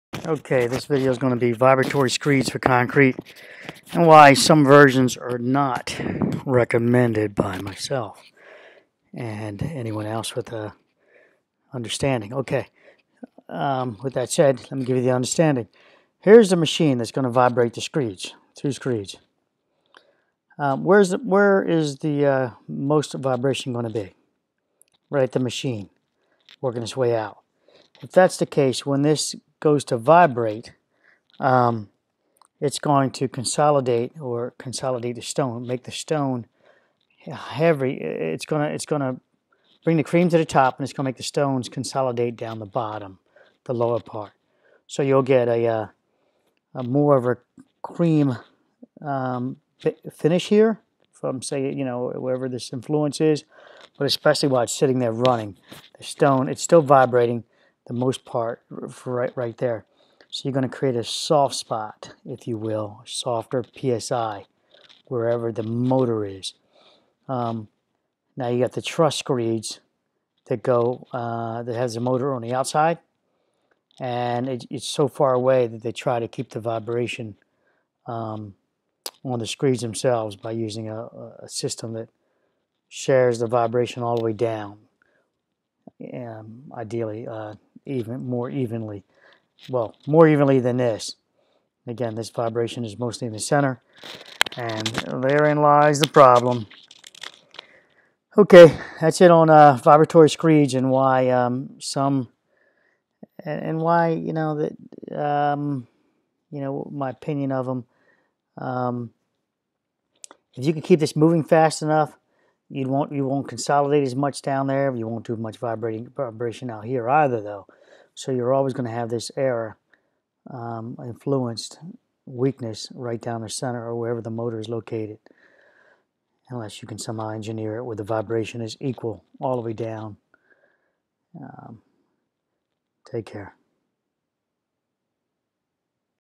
concrete vibrator screeds and how to choose one that is right for the job
Here is my wishlist if you care to go that direction Concrete vibrator screeds and how to purchase, this will explain why screeds, machine operated, can be an issue and weaken your concrete.
This is because, the vibration is vibrating and consolidating your stones towards the bottom, making the top layer - thicker with ''cement" and allowing for less of a bond of cement around the stones that are below the vibrated area...
Power screeds or concrete vibrators will cause your stones to set towards the bottom of the mix... This is NOT calculated by the concrete plants design...therefore, you will NOT have the same PSI concrete as specified by the supplier.